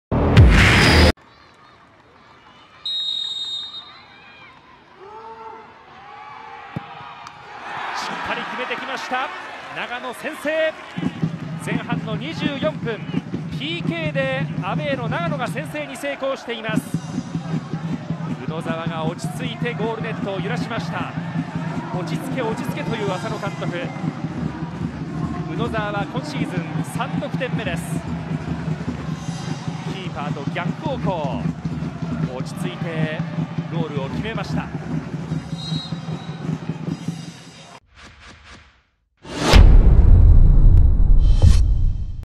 【公式】ゴール動画：宇野沢 祐次（長野）24分 Ｙ．Ｓ．Ｃ．Ｃ．横浜vsＡＣ長野パルセイロ 明治安田生命Ｊ３リーグ 第27節 2017/10/14
宇野沢 祐次（長野）が自ら獲得したPKをゴール左隅に蹴り込み、シーズン3点目となる先制弾を挙げる！明治安田Ｊ３のゴール動画を公開中！
2017年10月14日（土）に行われた明治安田生命Ｊ３リーグ 第27節 YS横浜vs長野のゴール動画です。